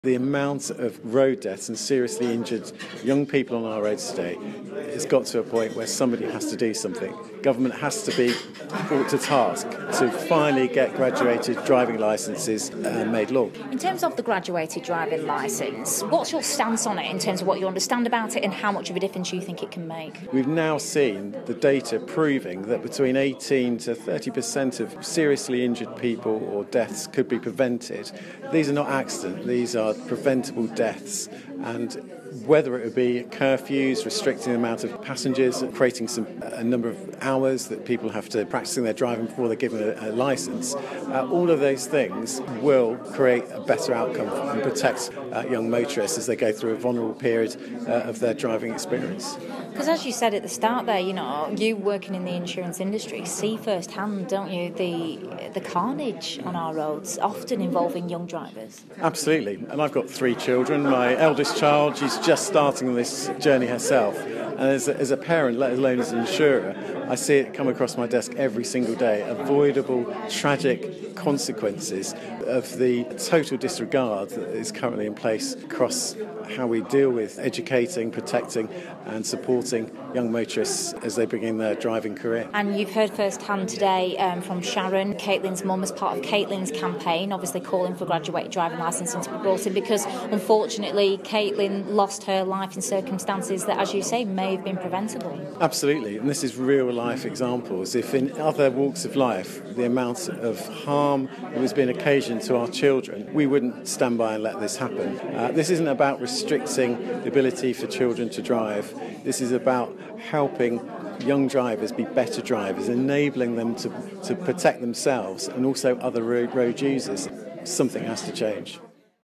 LV Insurance on Graduated Driving Licenses
LV insurance are one of a number of unsrance companies backing our Caitlin's Campaign calling on the Government to introduce a Graduated Driving License.